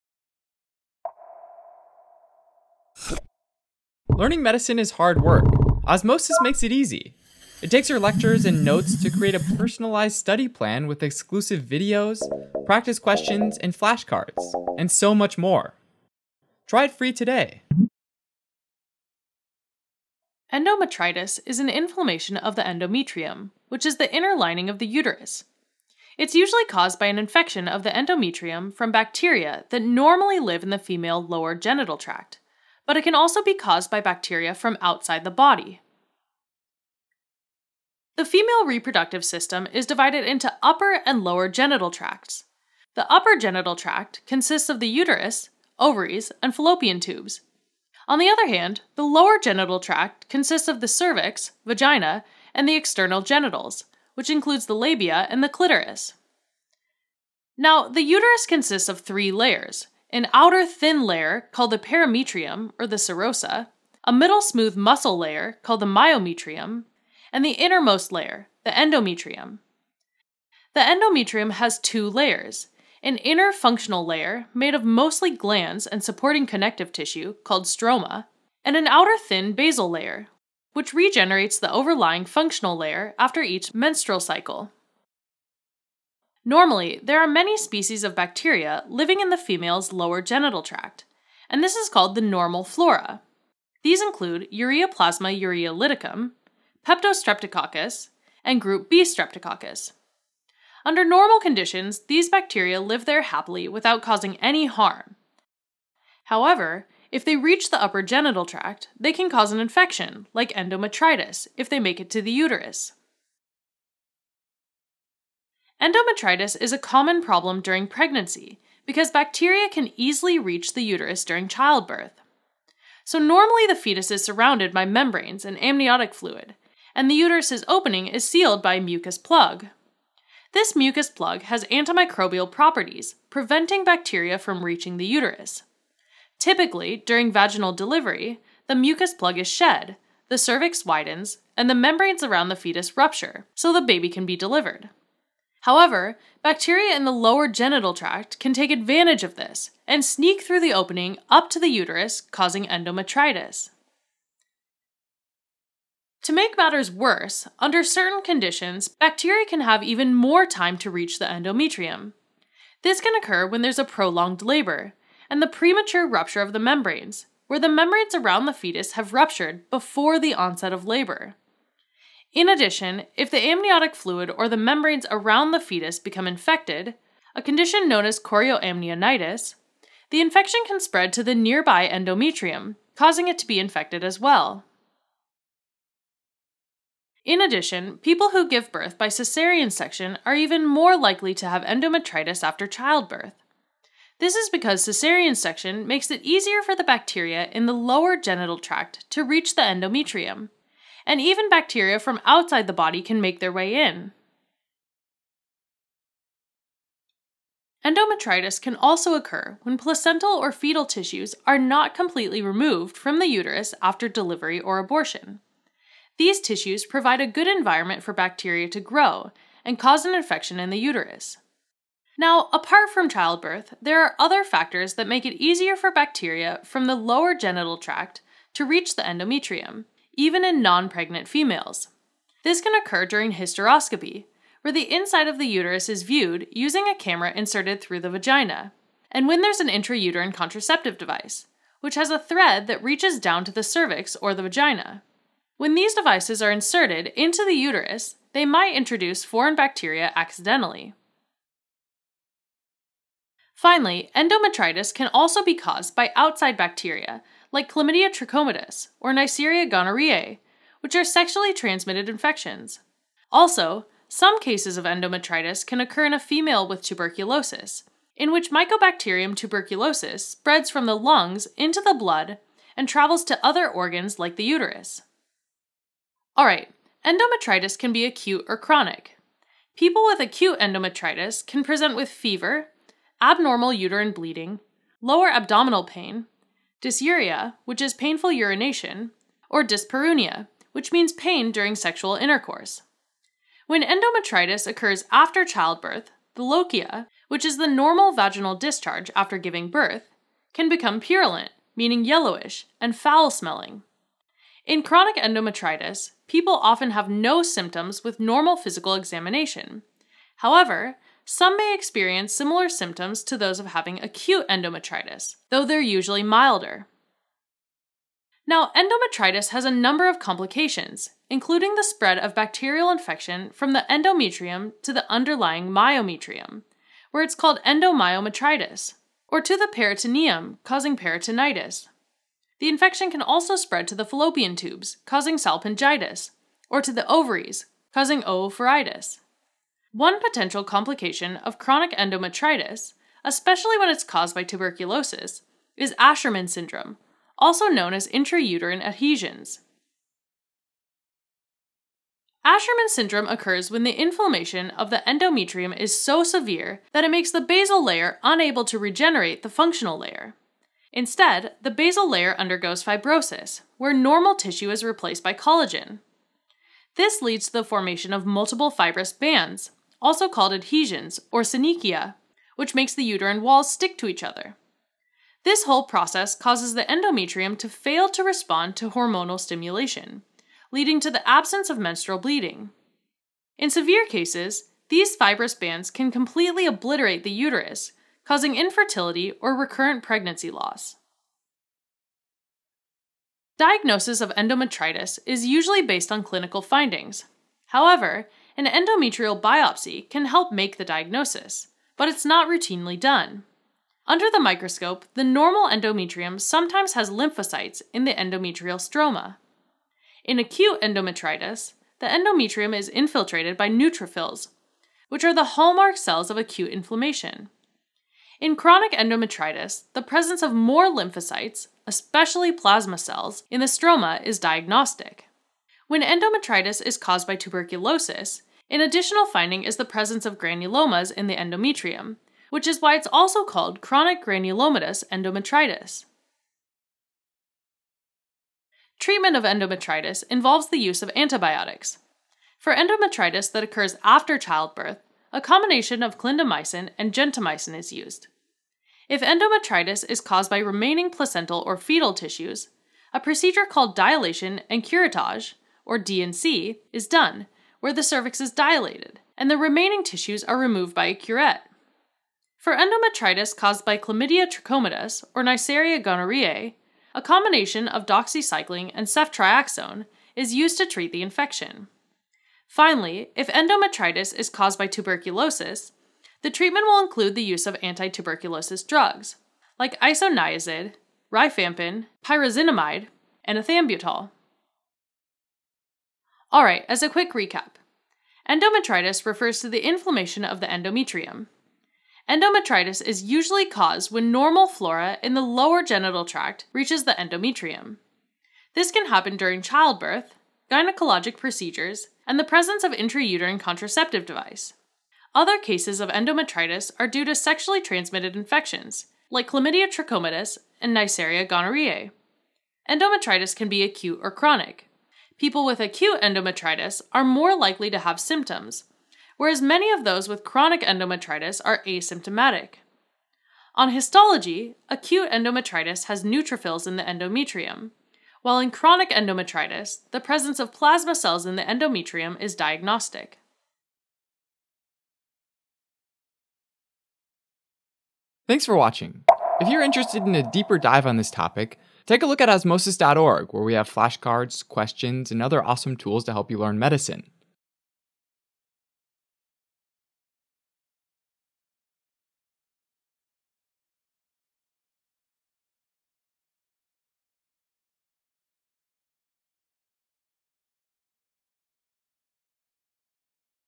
Endometritis - causes, symptoms, diagnosis, treatment, pathology
What is Endometritis? Endometritis is an inflammation of the endometrium, which is the inner lining of the uterus. Find our full video library only on Osmosis